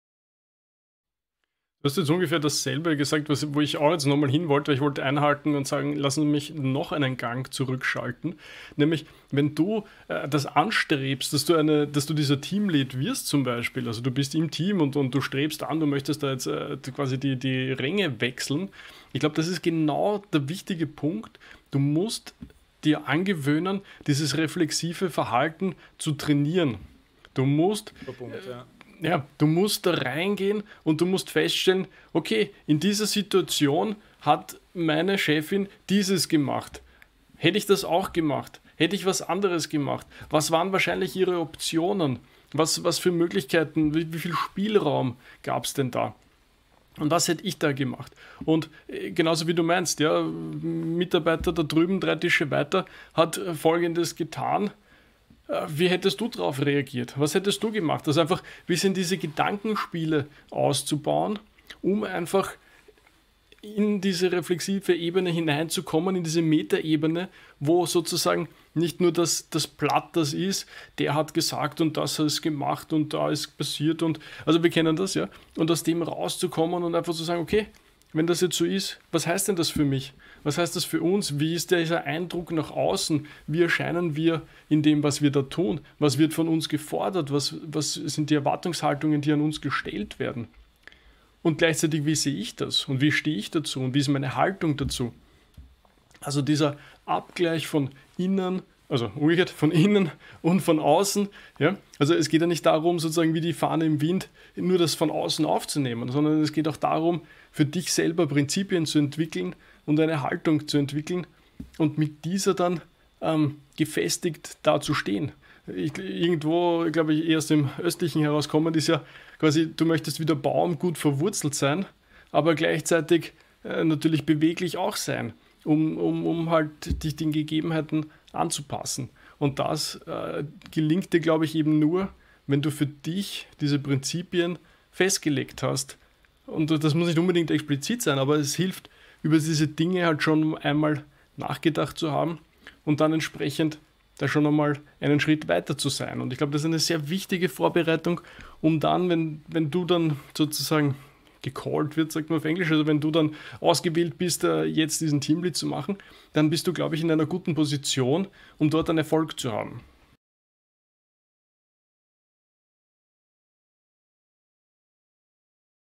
Wie wachse ich als Führungskraft in meine neue Rolle?
In diesem kurzen Clip spricht Flo über den Start als neue Führungskraft und wie wichtig der Perspektivenwechsel und der Reflexionsprozess ist.